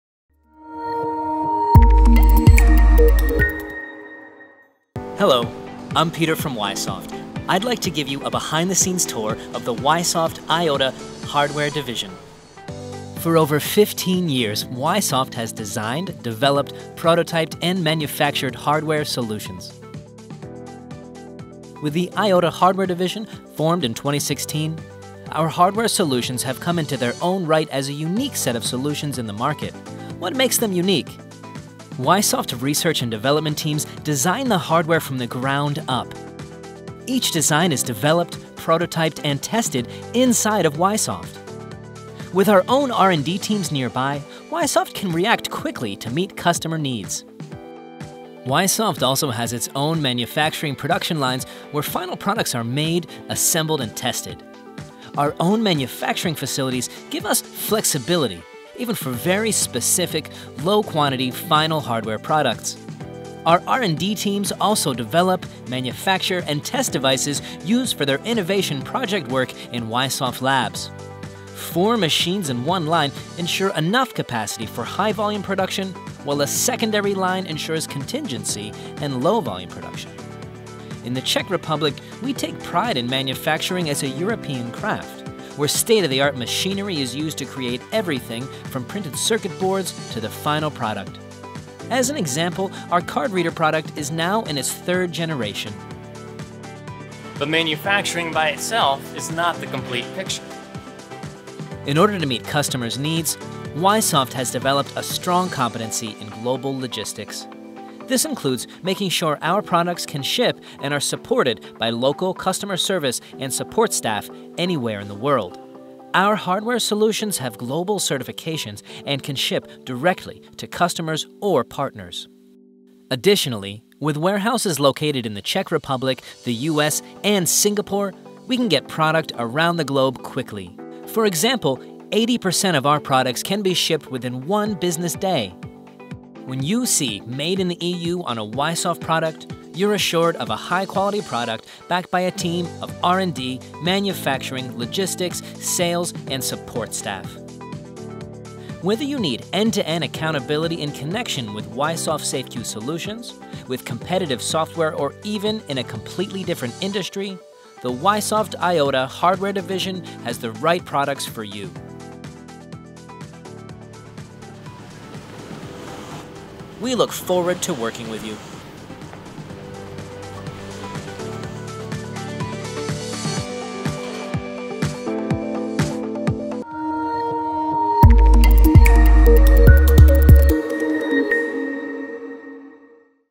YSoft HARDWARE division - Overview - 2018
The HARDWARE division is a growing area of Y Soft’s business. In addition to manufacturing and testing YSoft SAFEQ companion products, 3D printers, among others, the HARDWARE division supports Y Soft teams by creating prototypes on potential new products.